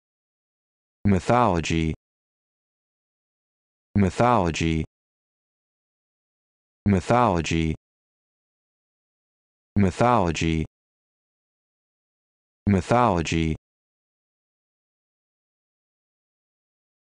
How to Pronounce Mythology
This video shows you how to pronounce Mythology